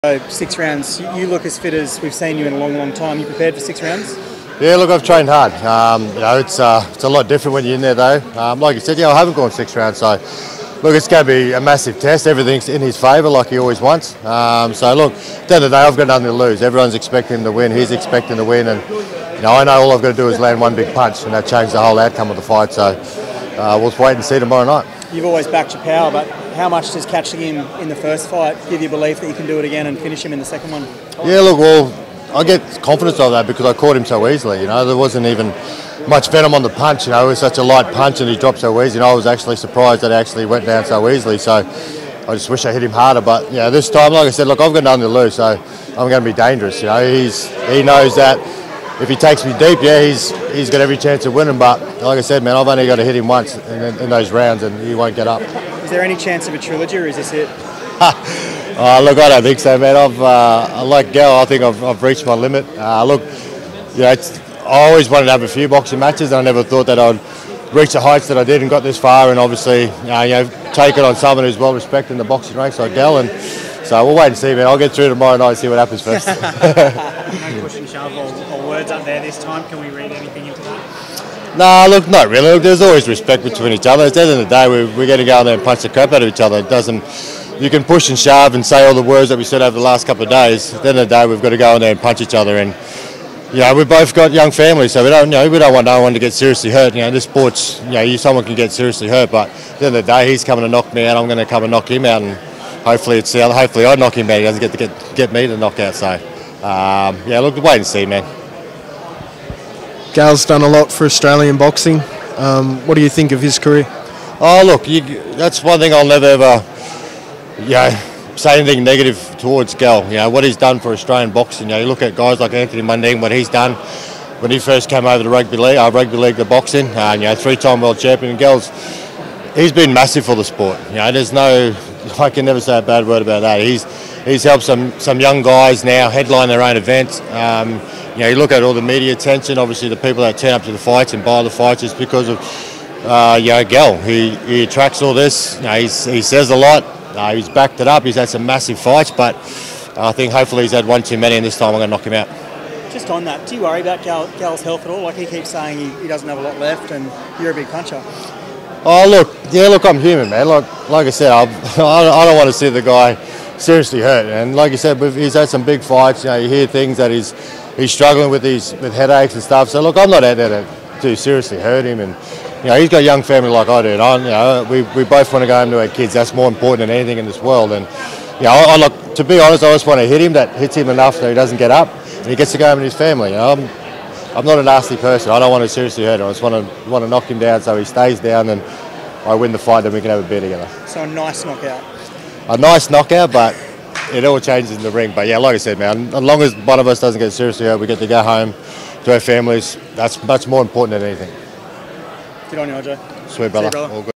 ‘HE’S BEEN MASSIVE FOR AUSTRALIAN BOXING’ | JUSTIN HODGES PAYS TRIBUTE TO PAUL GALLEN!
Justin Hodges speaks after the WEIGH IN for his upcoming rematch against Paul Gallen, set to take place on Wednesday Night in Sydney.